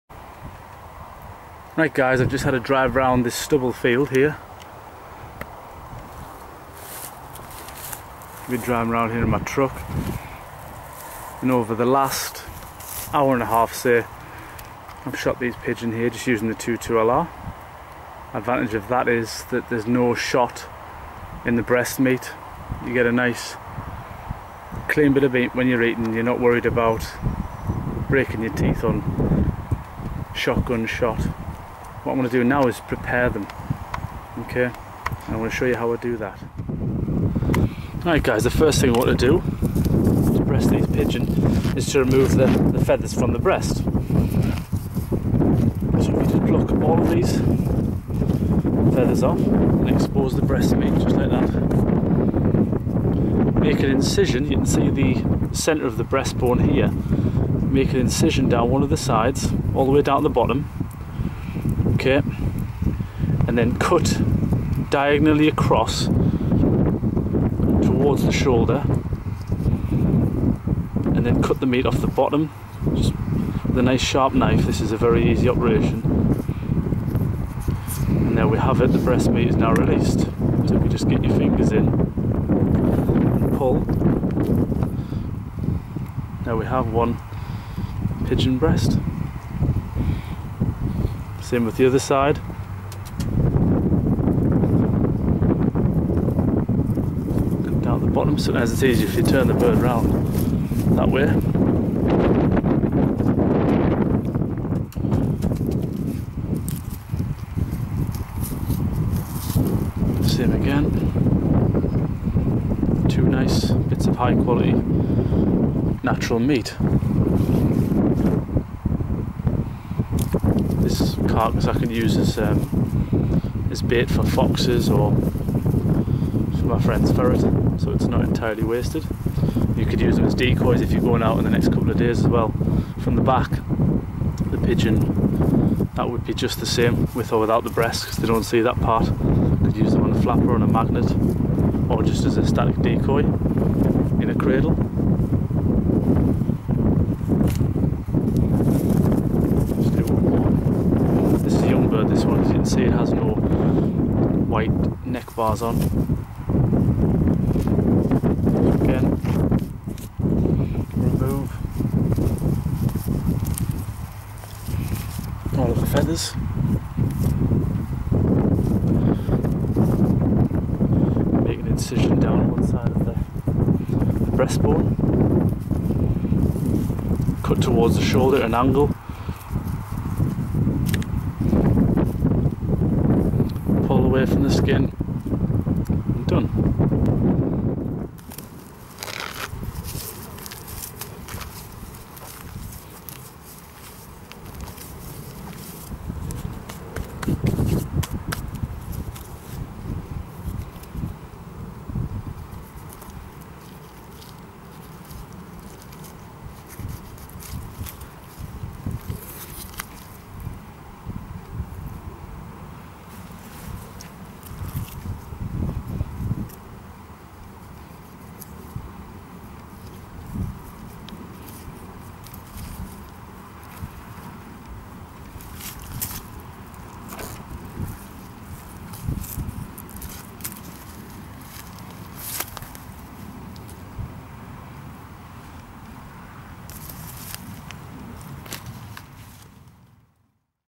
Wood pigeon breasting in the field
Clips showing the preparation of wild wood pigeon.